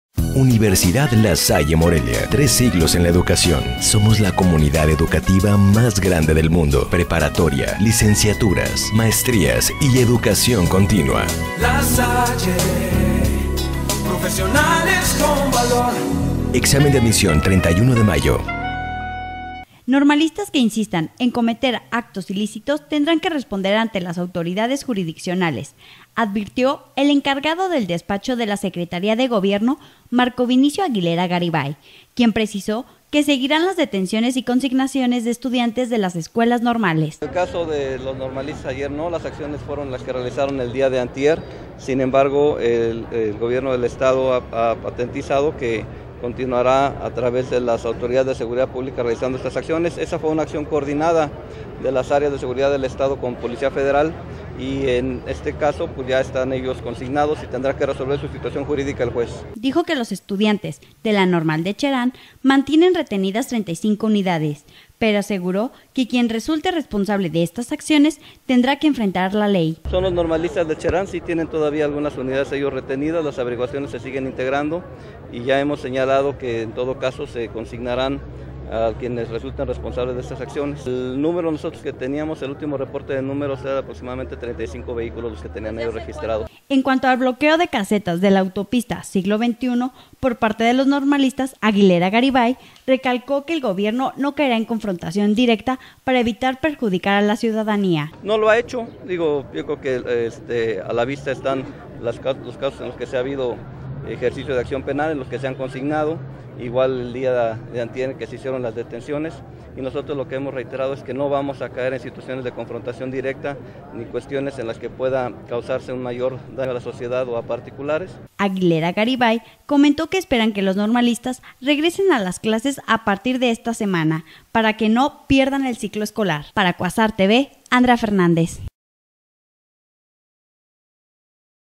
Normalistas que cometen ilícitos tienen que enfrentar la ley
Normalistas que insistan en cometer actos ilícitos tendrán que responder ante las autoridades jurisdiccionales, advirtió el encargado del despacho de la Secretaría de Gobierno, Maco Vinicio Aguilera Garibay, quien precisó que seguirán las detenciones y consignaciones de estudiantes de las escuelas normales...